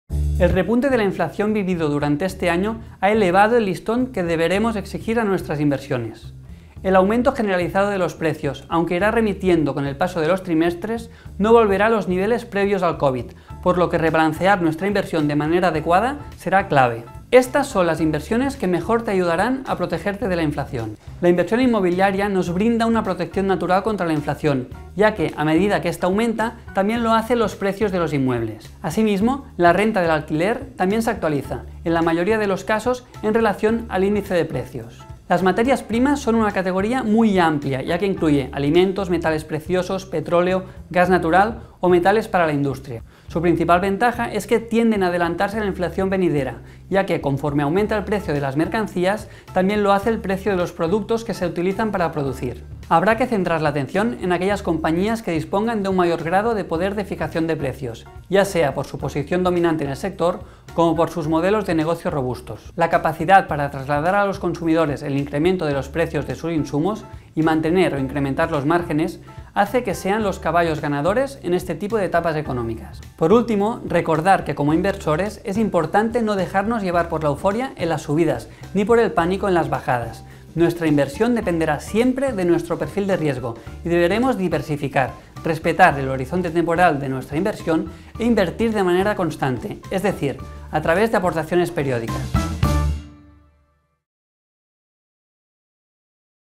Descubre cómo invertir para protegerte de la inflación - BANCO SABADELL
En el vídeo te explicamos cómo debes invertir para protegerte de la inflación.